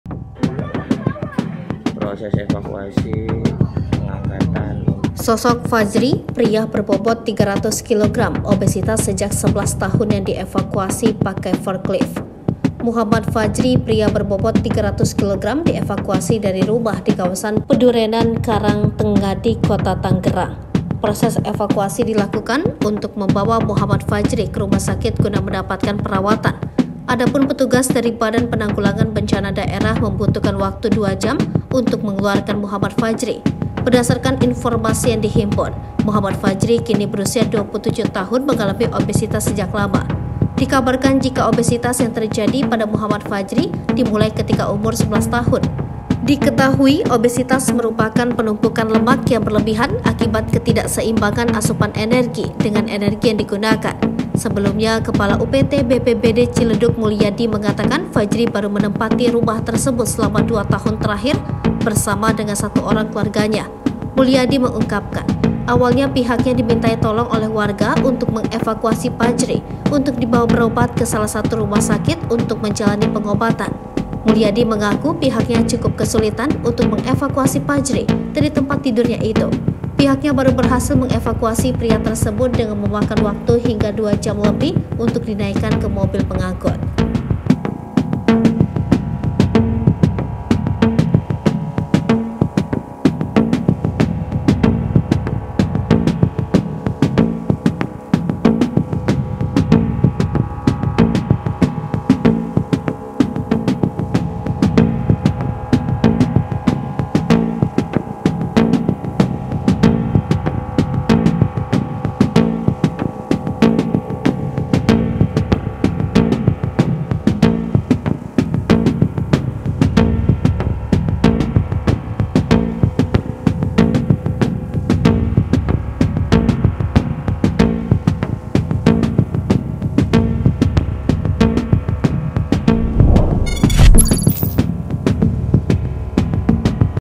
Sosok Fajri Pria Berbobot 300 Kg Obesitas Sejak 11 Tahun yang dievakuasi Pakai Forklift
Muhammad Fajri pria berbobot 300 kg dievakuasi dari rumah di kawasan Pedurenan, Karang Tengadi kota Tangerang.

Proses evakuasi dilakukan untuk membawa Muhammad Fajri ke rumah sakit guna mendapatkan perawatan.

Adapun petugas dari Badan Penanggulangan Bencana Daerah (BPBD) membutuhkan waktu 2 jam untuk mengeluarkan Muhammad Fajri.
Lihat informasi terkini bersama kami, di:

Palembang.tribunnews.com